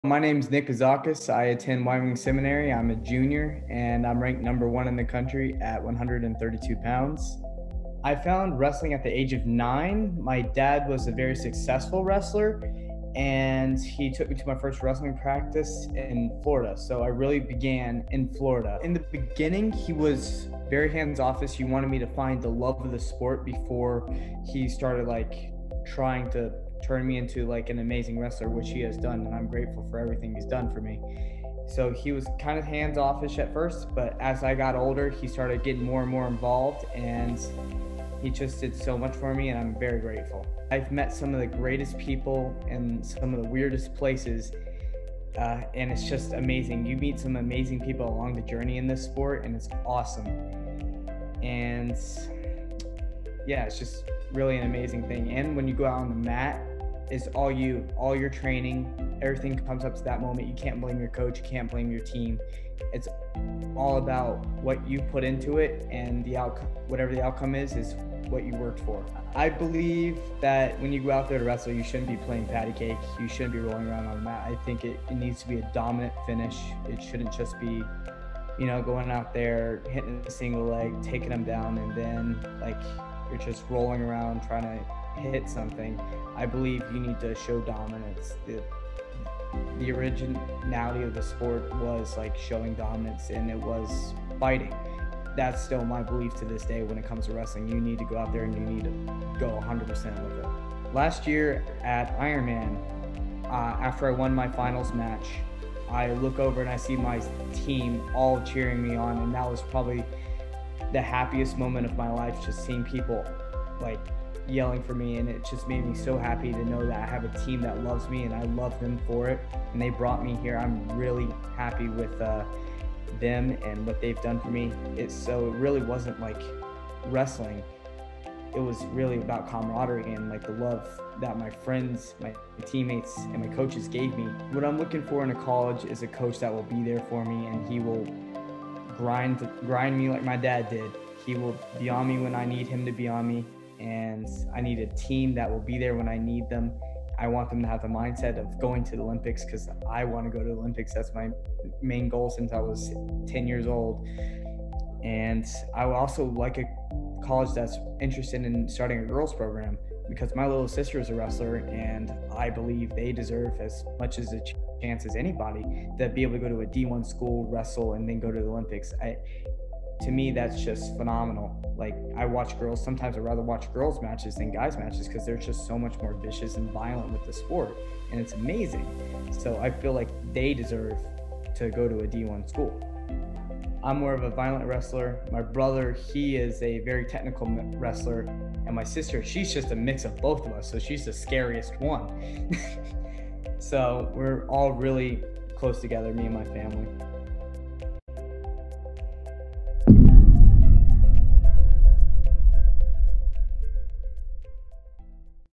Prep Spotlight: Meet top-ranked Nic Bouzakis
Wyoming Seminary junior Nic Bouzakis is ranked No. 1 in the Trackwrestling rankings at 132 pounds.